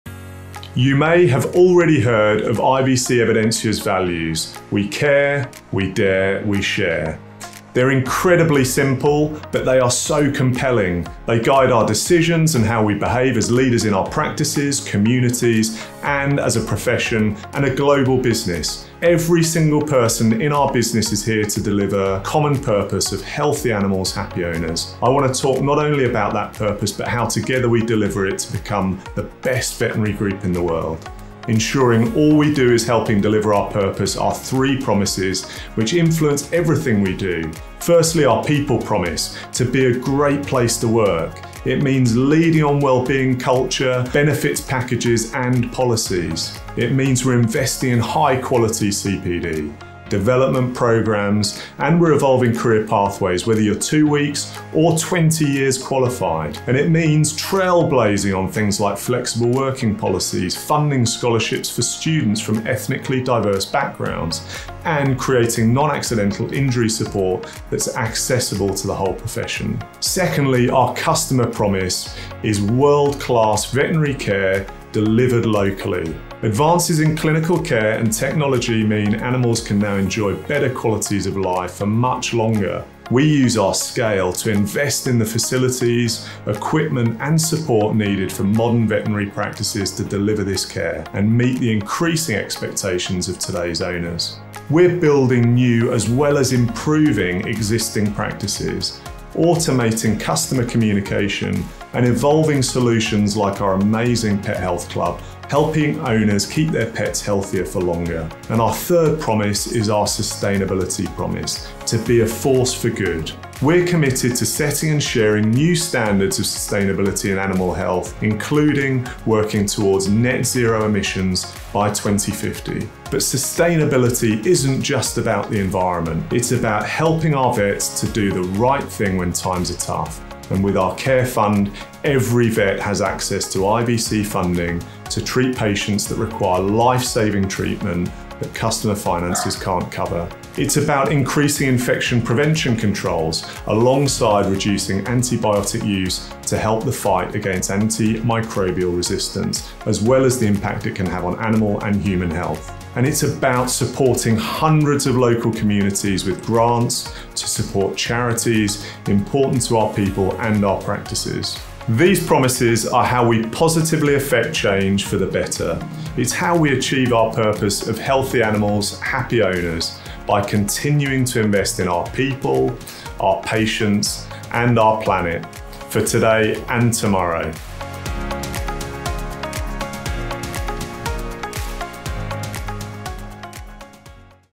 IVC Evidensia Ireland
CEO Duncan Phillips talks about our company values, how they guide everything we do and help us to deliver our purpose of Healthy Animals, Happy Owners.

Discover the huge range of support and investment that goes into propelling our three promises to our customers, our people and our planet. It's how we will become the best veterinary group in the world, delivering world class veterinary care locally, from